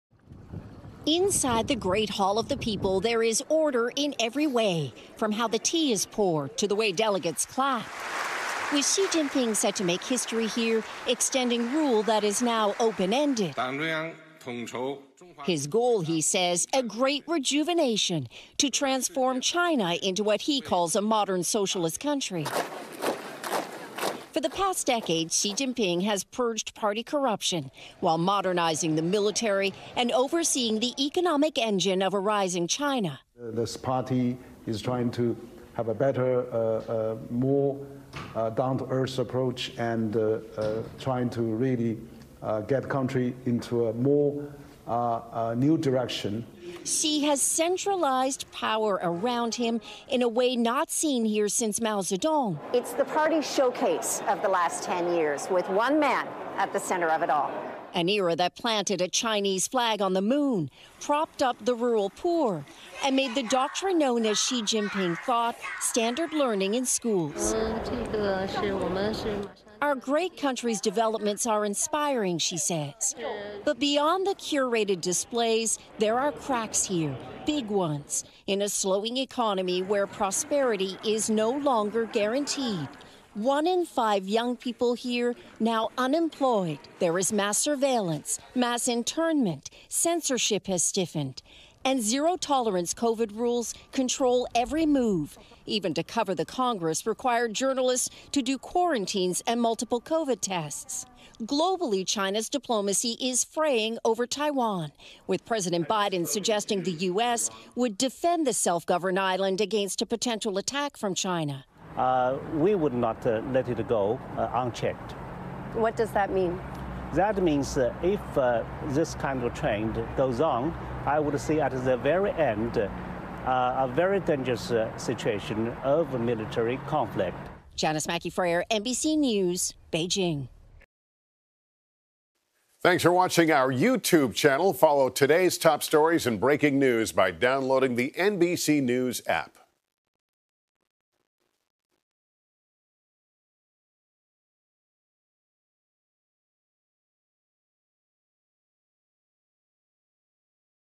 China's Communist Party Highlights Xi Jinping's Power
China’s President Xi Jinping is set to make history within the country by extending his rule which is now open-ended. Xi’s goal in the “Great Rejuvenation” is to transform China into what he claims is a modern socialist nation. NBC News' Janis Mackey Frayer reports on why this highlights how his power is designed to be a show of force.